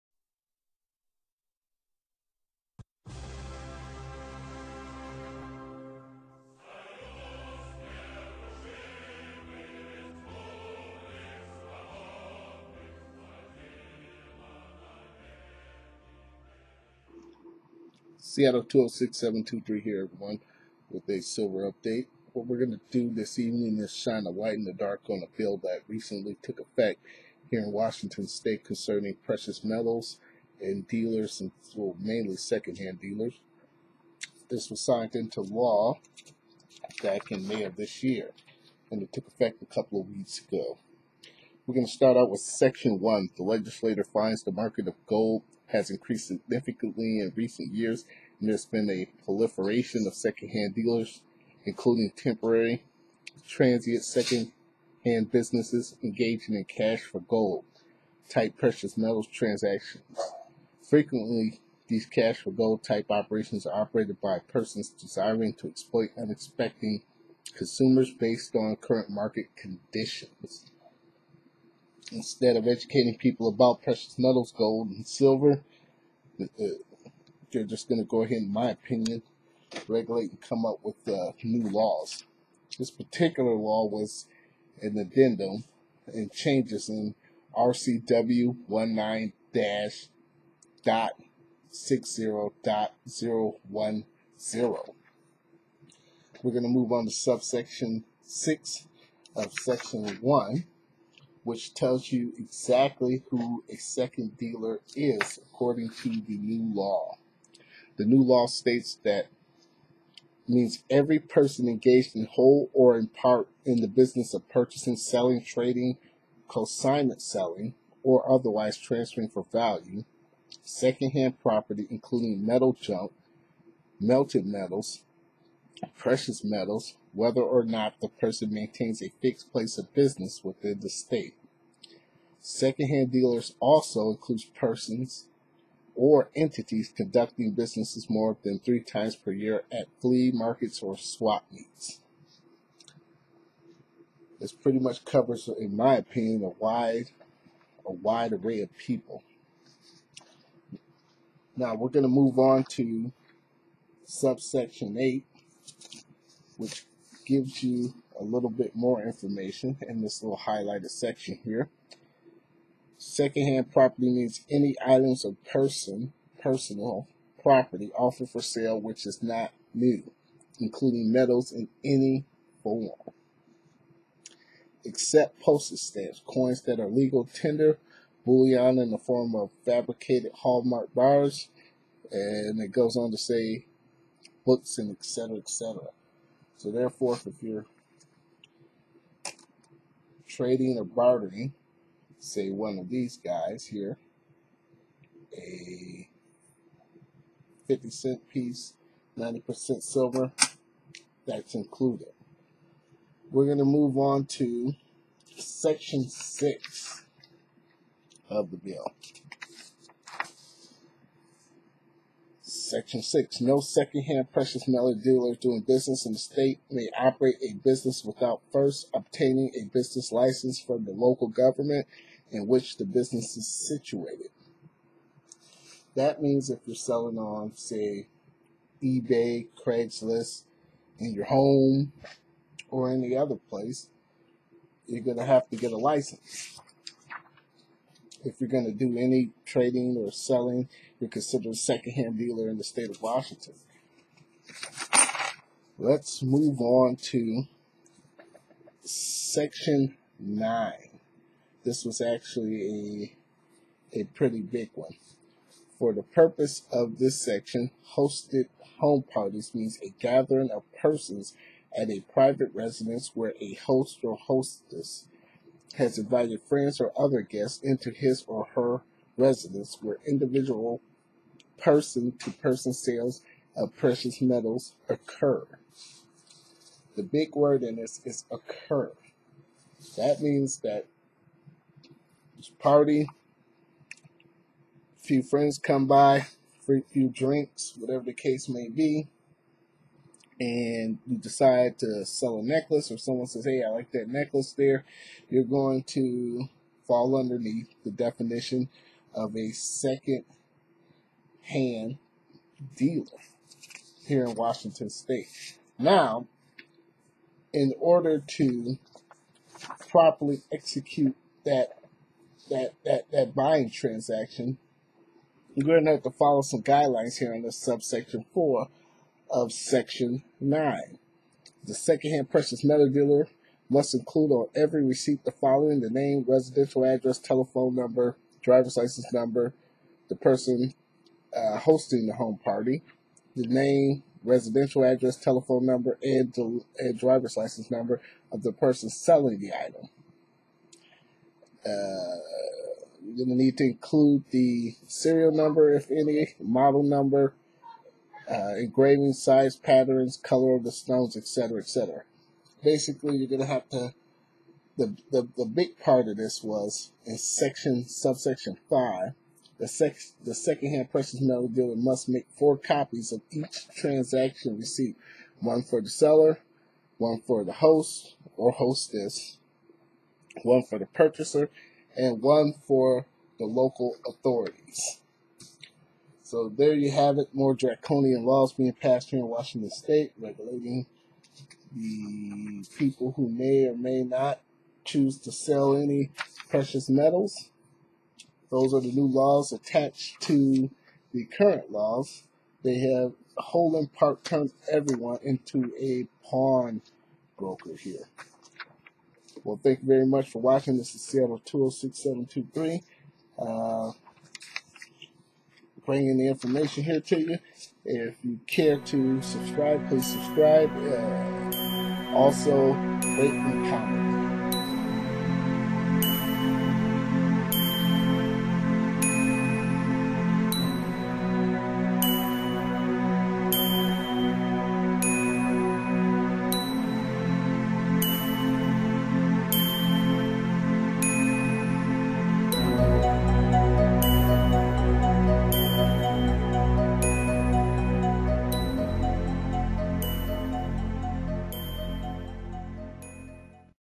Washington State **Restricts** GOLD & SILVER SALES!!!!
Question? Are You Selling Gold or Silver in Washington State? If the answer is yes, and you don't meet the requirements your going to jail!!!

If you are selling gold or silver in Washington State; your going to need to apply with all the regulations below. If you don't your going to Jail folks. The link below is the bill from the Washington State Gov Mint bellow.

You get caught selling the first time

1ST Offense Gross Misdemeanor "Up to 12 Months in the County"
2ND Offense Your a Felon "Welcome too the Big House"

Washington State New Law Governing Gold Silver Sales
Link of Bill here

Zero Hedge

Correction added legal tender coins are exempt